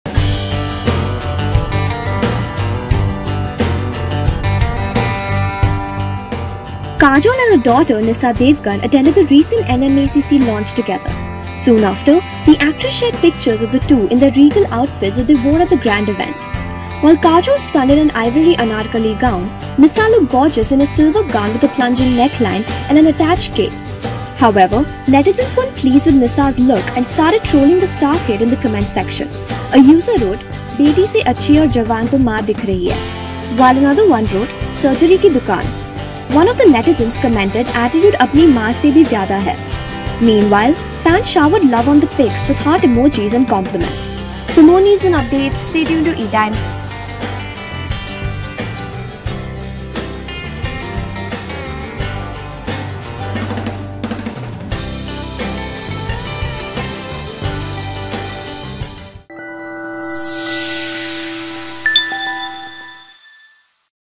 Nysa Devgan poses in a bold dress with mother Kajol, gets trolled
Kajol and her daughter Nysa Devgan attended the recent NMACC launch together. Soon after, the actress shared pictures of the two in their regal outfits that they wore at the grand event. While Kajol stunned in an ivory anarkali gown, Nysa looked gorgeous in a silver gown with  a plunging neckline and an attached cape. However, netizens weren’t pleased with Nysa’s look and started trolling the star kid in the comments section. A user wrote, ‘Beti se acchi or jawaan to maa dikh rhi’, while another one wrote, ‘Surgery ki Dukaan’. One of the netizens commented, ‘Attitude apni maa se bhi jyada h’. Meanwhile, fans showered love on the pics with heart emojis and compliments.